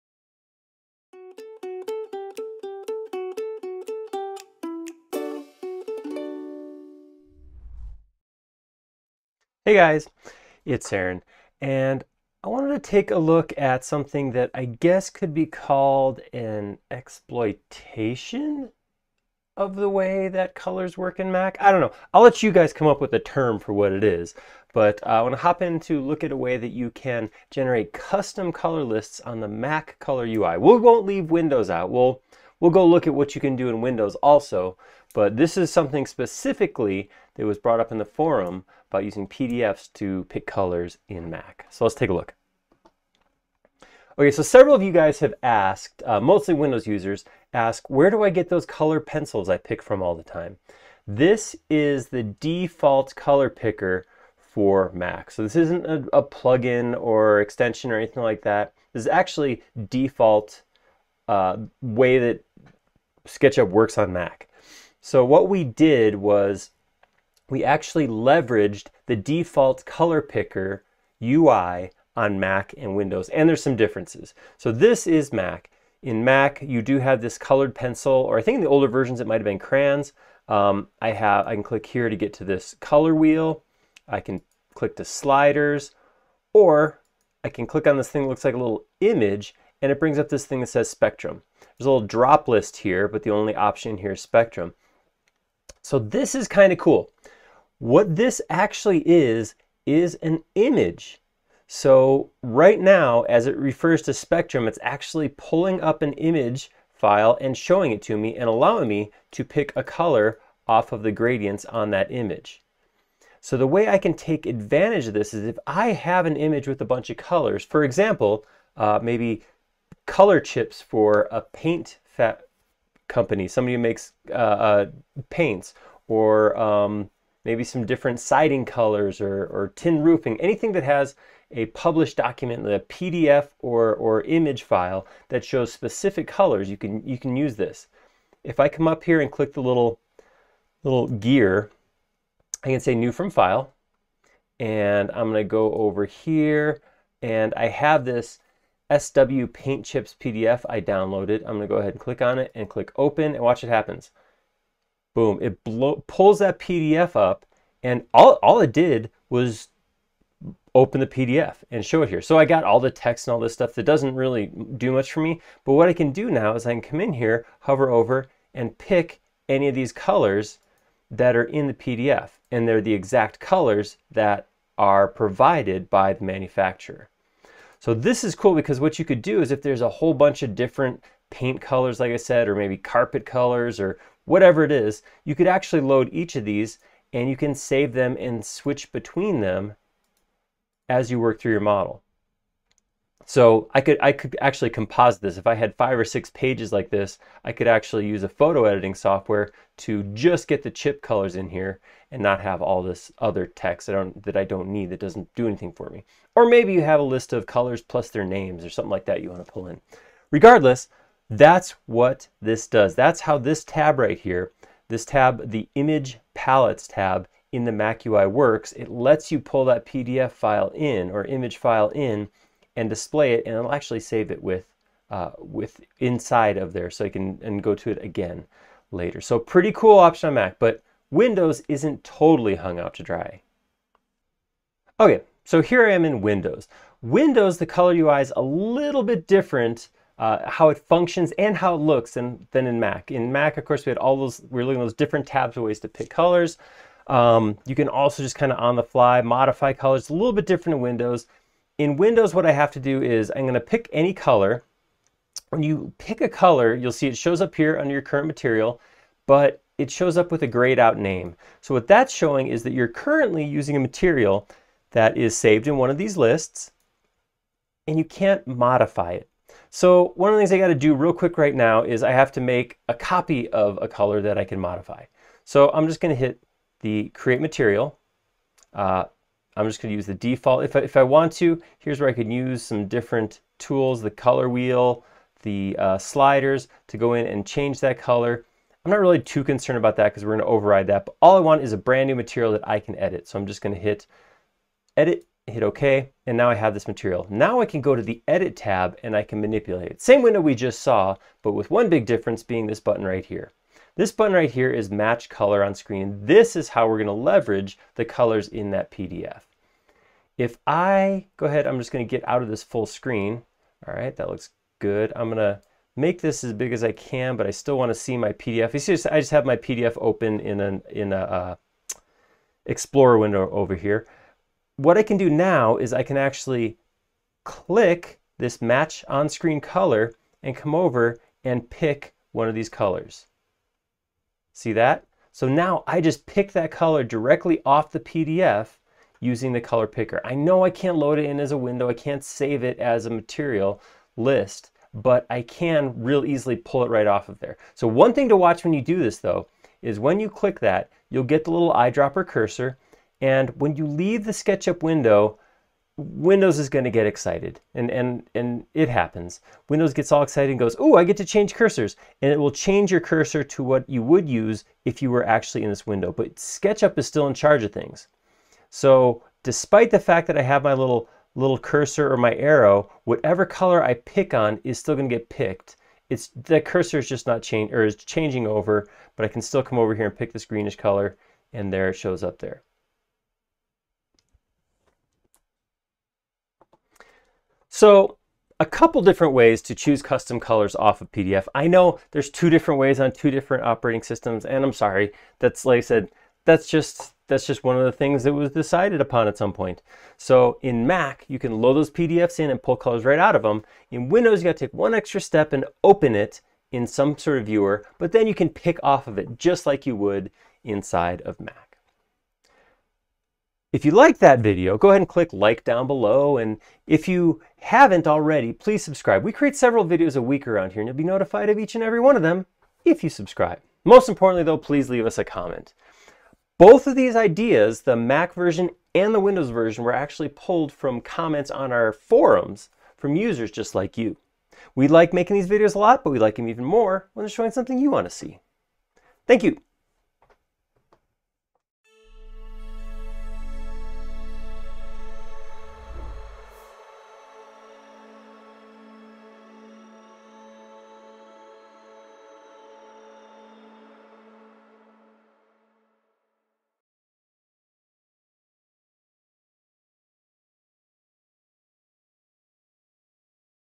Picking colors from a PDF - Skill Builder
Today we're taking a look at a way you can generate a custom color list with the Mac UI. Thanks to our forum users for this great idea!